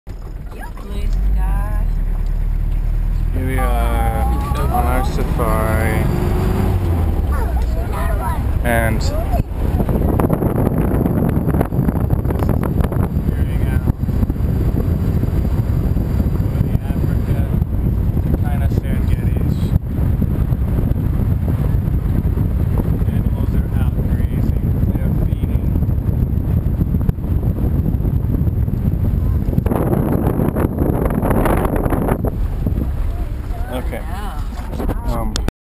What a typical safari looks like
Here we are beginning our safari early in the morning. We were in the van by 6:30 because animals obviously do not sleep in.